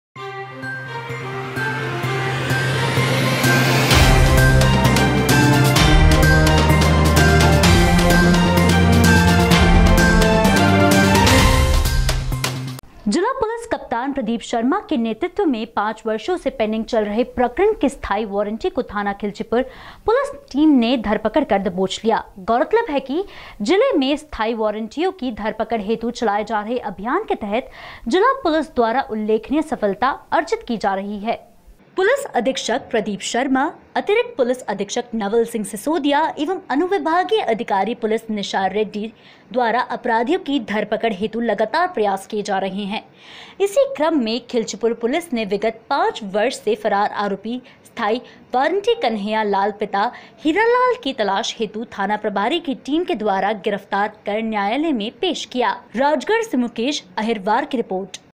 राजगढ - पुलिस ने फरार आरोपी को धर-दबोचा
जिला पुलिस कप्तान प्रदीप शर्मा के नेतृत्व में 05 वर्षो से पेंडिंग चल रहे प्रकरण के स्थाई वारण्टी को थाना खिलचीपुर पुलिस टीम ने धरपकड कर दबोच लिया। गौरतलब है कि जिले में स्थाई वारण्टीओं की धरपकड हेतु चलाए जा रहे अभियान के तहत जिला पुलिस द्वारा उल्लेखनीय सफलता अर्जित की जा रही है ।
पुलिस अधीक्षक प्रदीप शर्मा, अतिरिक्त पुलिस अधीक्षक  नवल सिंह सिसोदिया एंव अनुविभागीय अधीकारी पुलिस निशा रेड्डी द्वारा अपराधियों की धडपकड हेतु लगातार प्रयास कियें जा रहे है इसी क्रम मे खिलचीपुर पुलिस ने विगत 05 वर्ष से फरार आऱोपी स्थाई वारण्टी कन्हैया लाल  पिता हीरालाल की तलाश हेतु  थाना प्रभारी की टीम के द्वारा गिरफ्तार कर न्यायलय में पेश किया।